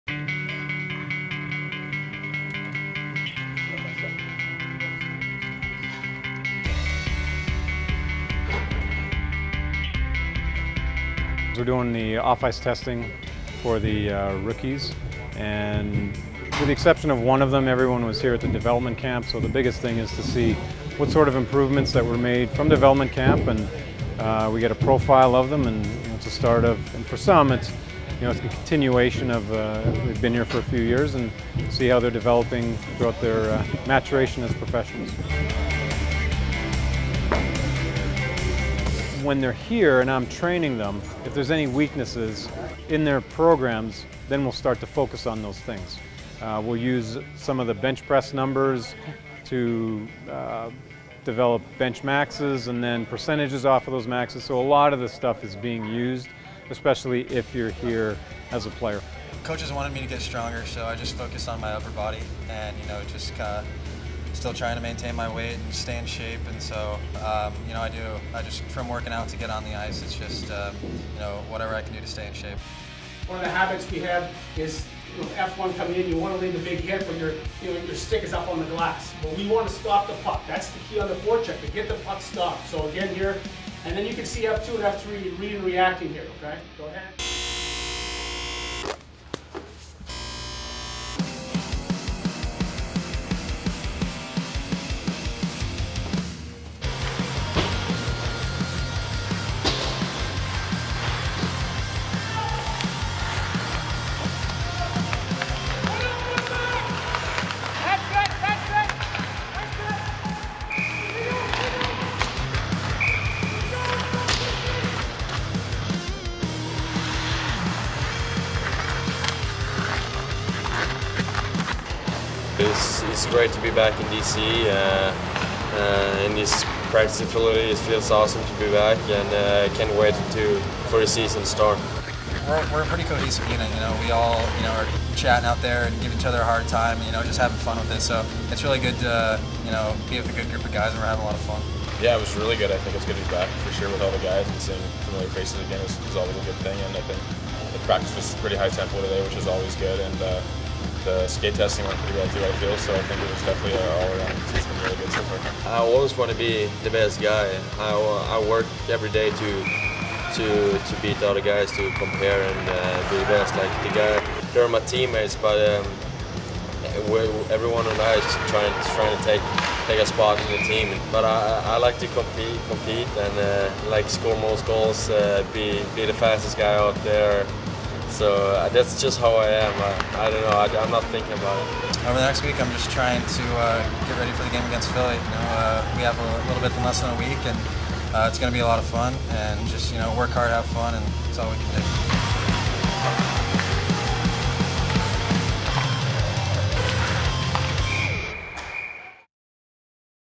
Behind the Scenes at CapsRookieCamp 9/12/14
Get an exclusive look at this season's CapsRookieCamp including off ice testing, film study and practice.